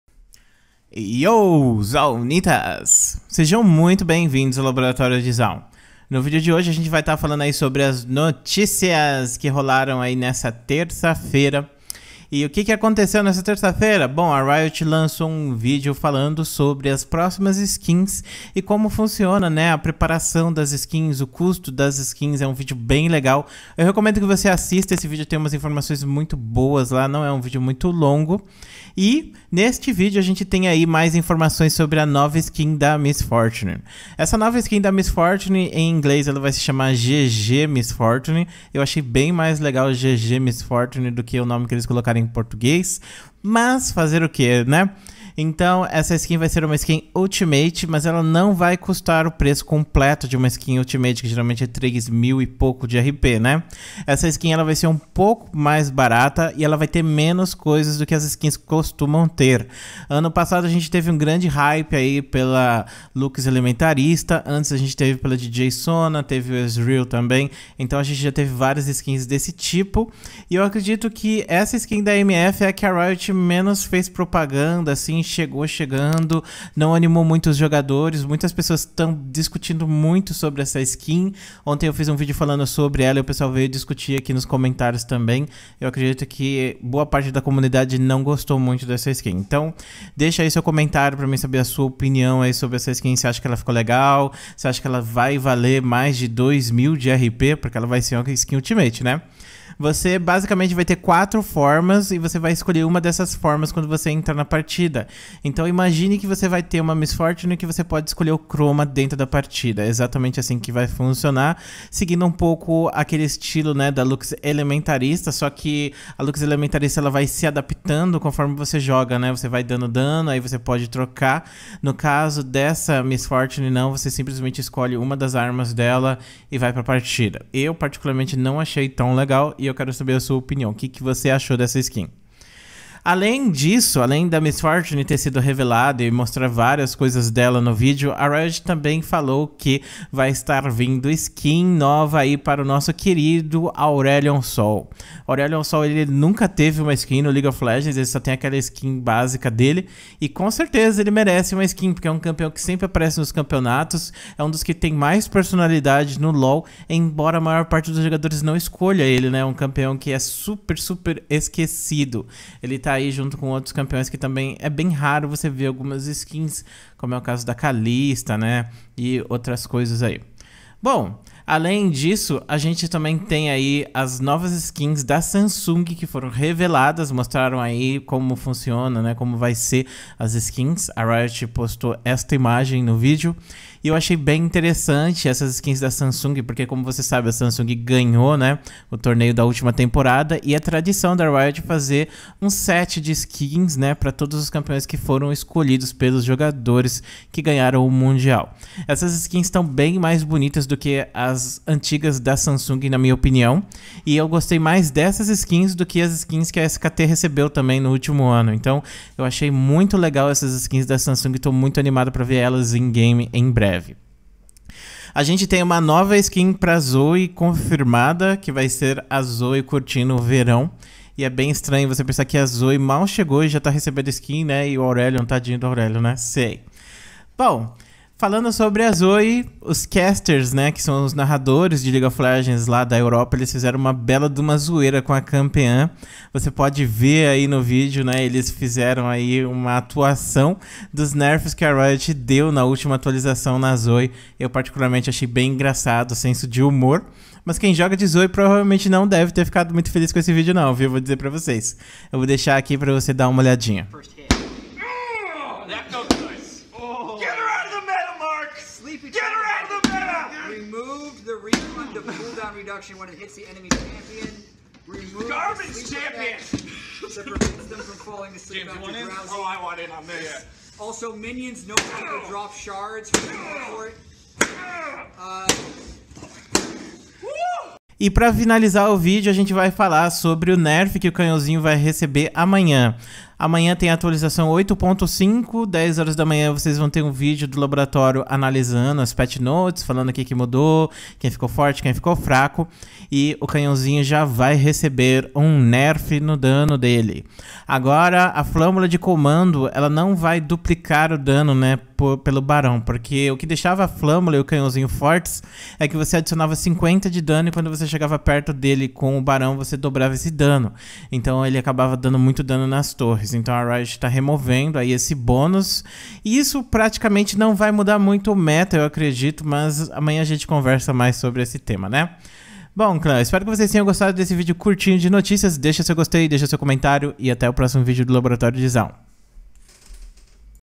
SKIN ULTIMATE DESAPONTOU? NOVAS SKINS DA SAMSUNG! REMAKE NA ZOE USANDO UM BASTÃO!
nossa essa skin da mf passou em branco hein clã?o que vocÊs acharam da ggmf ou gun goddess miss fortune, vingadora da exo alguma coisa

bom eu prefiro em ingles o nome GG MF é mais legal, e essas skins da samsung hein? mas a melhor parte do vídeo é o remake que deram na zoe